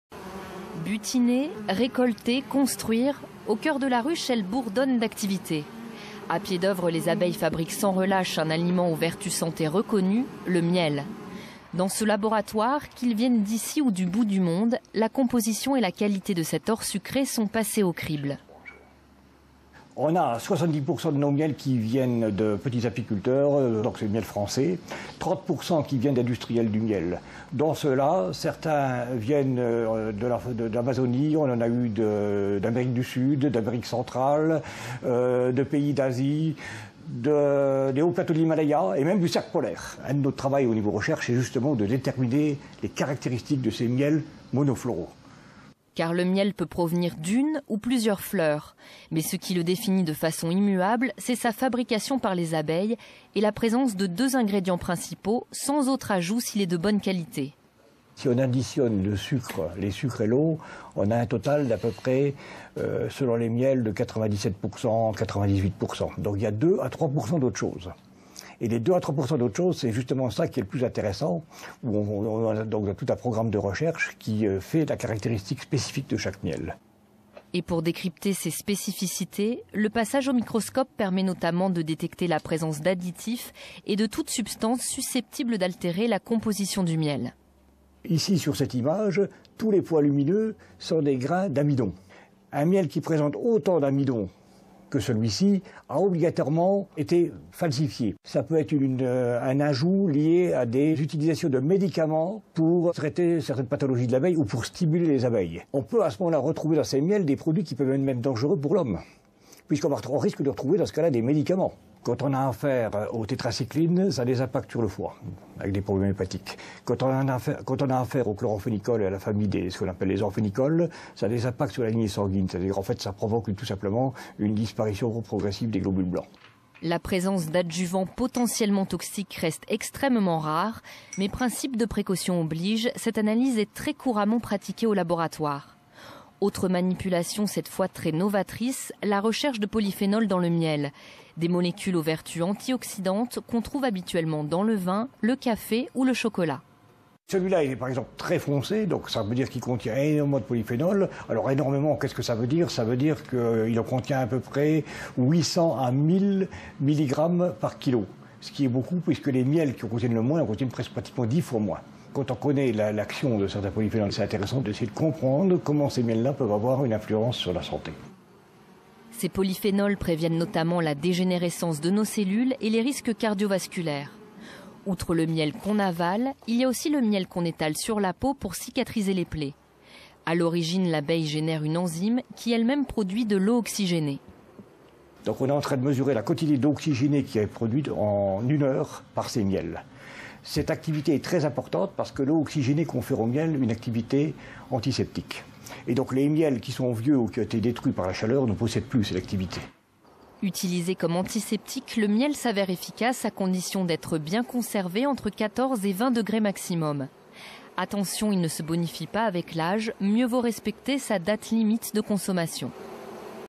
Etudes du miel au Centre d'Études Techniques Apicoles de Moselle
Le Centre d'Études Techniques Apicoles de Moselle étudie les miels reçus et vérifie qu'ils ne sont pas impropres à la consommation.